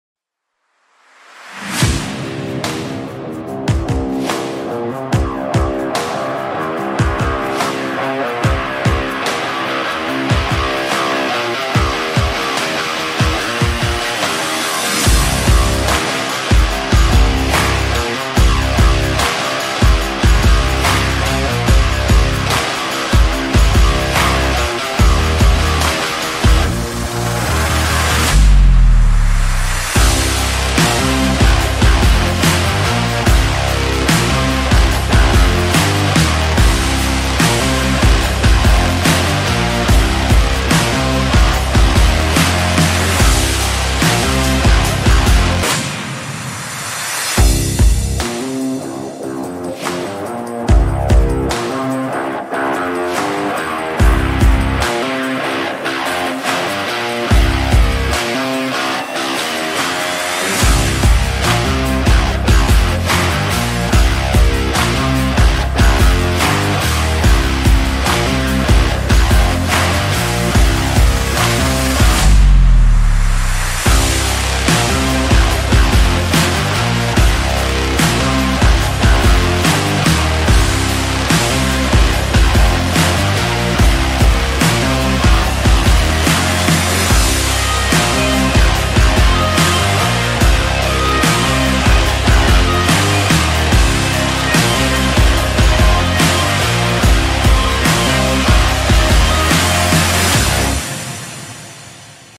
BMW 740Li shielded with PPF Instant Healing | Ultrakool
Dashing BMW 740Li shielded with PPF Instant Healing with Ultrakool

High-quality paint protection films offer shielding against scratches and rock chips, have self-healing properties, and prevent paint swirl formation. This is why many performance vehicle owners install them on the front of their cars, where bug debris and rock exposure happen most frequently.

Get your car detailed with the pros @ultrakoolcc